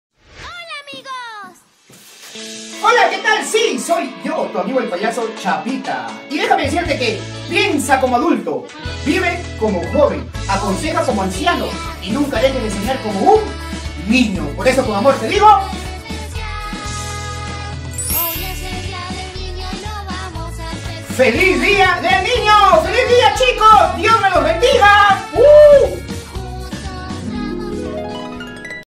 felíz día del niño
Un abrazo a todos mis amigos del Perú y del mundo DIOS me los proteja siempre 🙏🎊.

Con mucho cariño les desea su amigo el payaso CHAPITA .